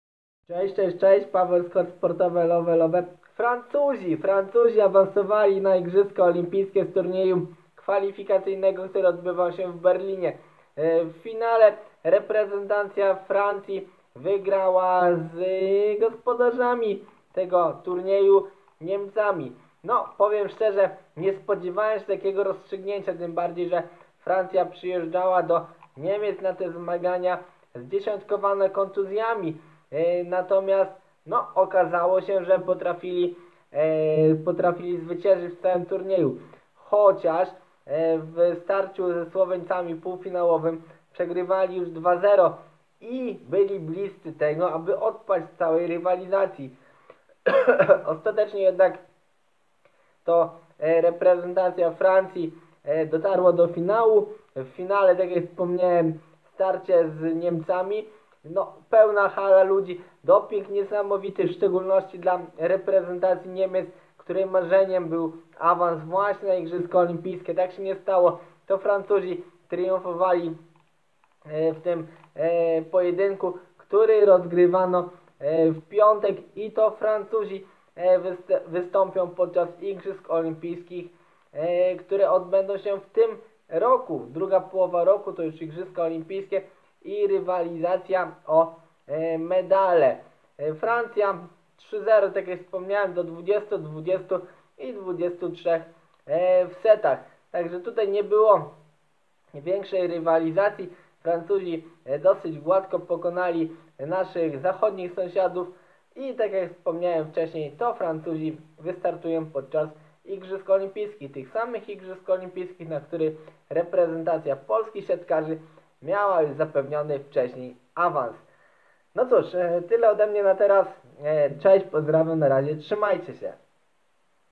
Francuzi wywalczyli miejsce na Igrzyska Olimpijskie!
Reprezentacja Francji okazała się najlepsza w męskich eliminacjach do Igrzysk Olimpijskich w Tokio. W finale pokonali Niemców. Turniej rozgrywano w Berlinie.